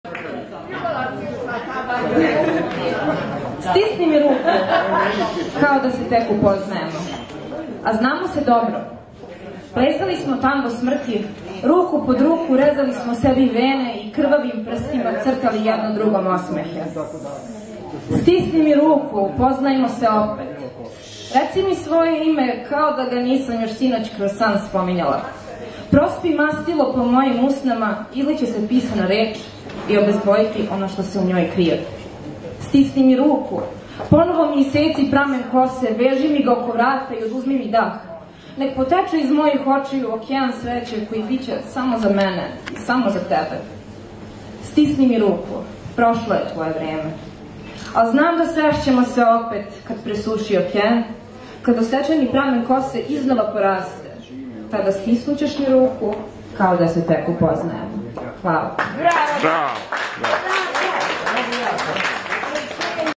Магично вече 13 # Дијана Јовановић
Дијана Јовановић, песникиња из Панчева на Магичној вечери 13 поезије, музике, плеса, перформанса... Улица бар, Балканска 13, Београд, 2019.